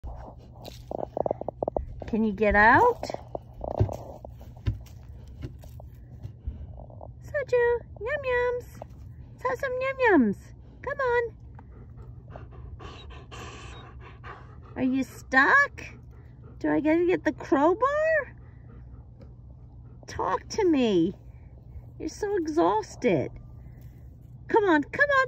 Sojou Are You Stuck?
Sojou is back digging her wolf den. A behaviour she has adopted as part of a pseudo or false pregnancy with her hormones all imbalanced. Time to get mama some help!